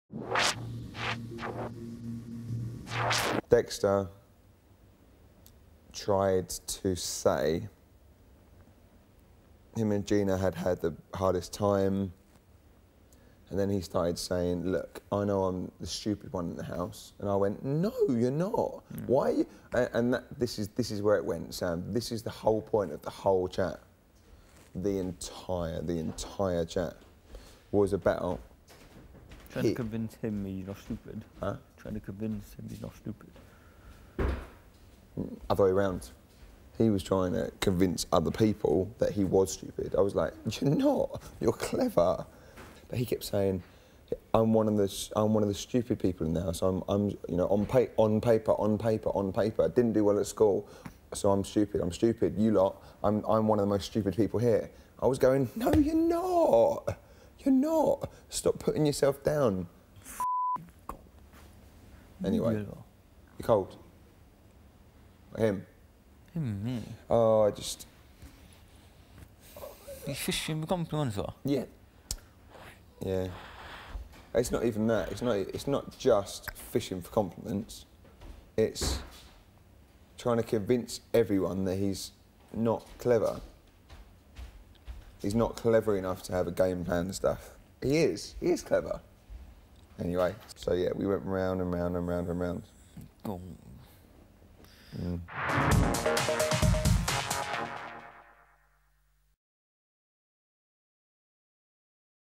Day 47: Callum dishes out the truth about last night
After consolidating Dexter over his concerns for being 'stupid' last night, Callum's slept on the conversation and awoke with a light bulb assumption.